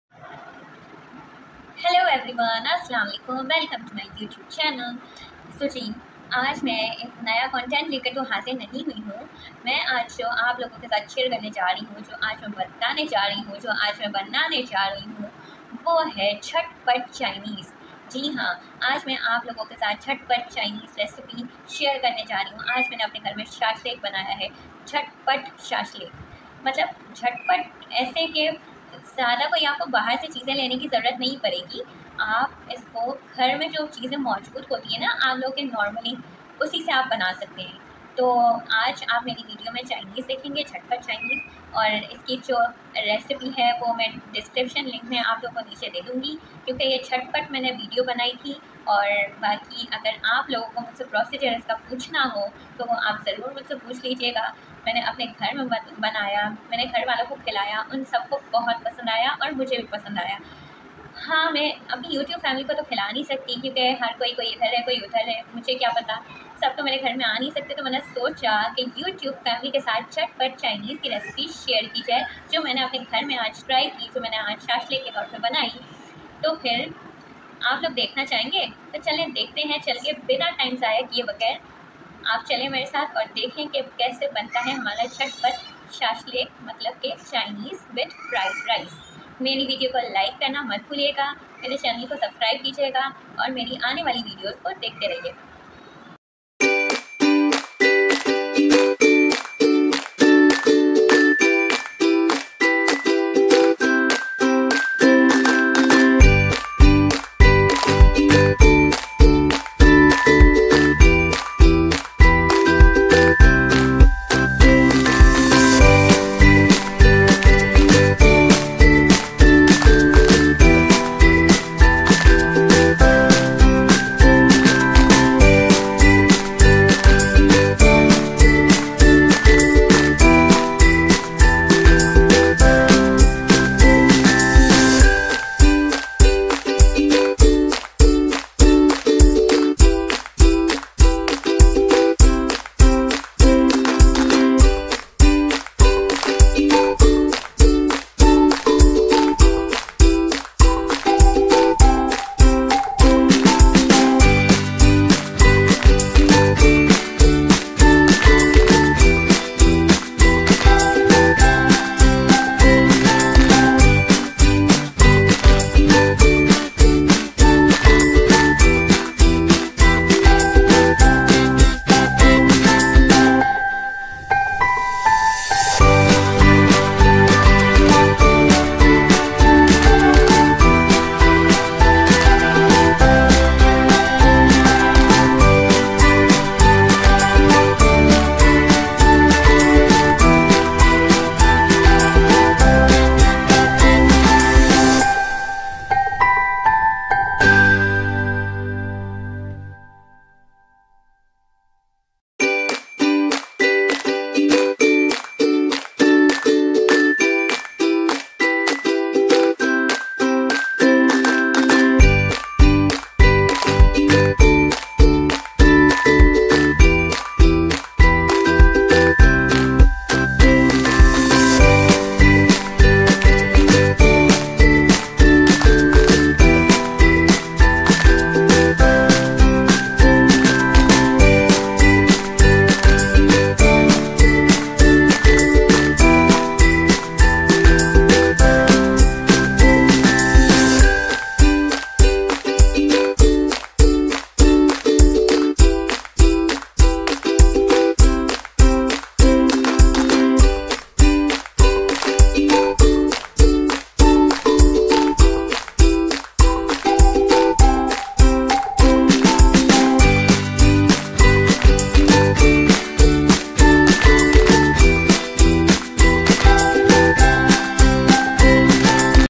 JHAT PAT SHASHLIK ghr pe banaya 😍 bara Maza aya👌
chicken shashlik recipe:
ingredients:
boneless chicken 1kg
soya sauce:8 tbsp
chilli sauce:8tbsp
black pepper:1tsp
white pepper:1tsp
adrak lasan paste:2 tbsp
chicken powder:1 tbsp
salt:1 tbsp
kuti hwi Laal Mirch:1 tbsp or 2 as u need
sirka:1 tbsp
marinate ur chicken for 1 hour kept in a frig
sauce preparing ingredients:
adrak lasan chopped
2 tbsp oil
ketchup 2 to 3 cups
soya sauce:6 tbsp
chilli sauce:6 tbsp
red chilli powder: 1 tbsp
salt: as u need it
black pepper: 1in a half tbsp
white pepper:2 tbsp
sugar:chutki si half tea spoon
sirka: 2 tbsp
cornflour: as u need it
water as u need it..

vegetables u need:
capsicum: 3
onion: 3
carrots: 2
tomatoes: 3
green onion: as u need it

if u need a full recipe so plz comment on my comment section